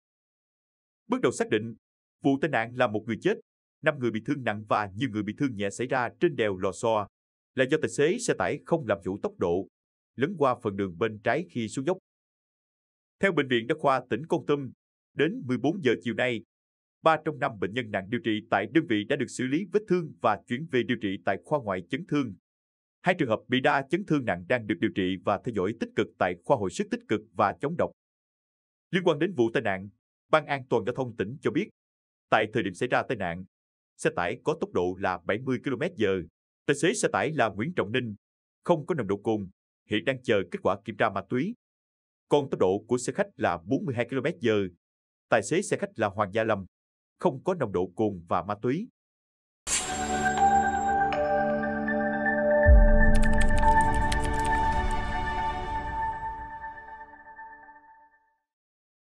Xảy ra tai nạn nghiêm trọng trên đèo Lò Xo vì tài xế không làm chủ tốc độ
Bước đầu xác định, Vụ tai nạn làm 1 người chết, 5 người bị thương nặng và nhiều người bị thương nhẹ xảy ra trên đèo Lò Xo, là do tài xế xe tải không làm chủ tốc độ, lấn qua phần đường bên trái khi xuống dốc.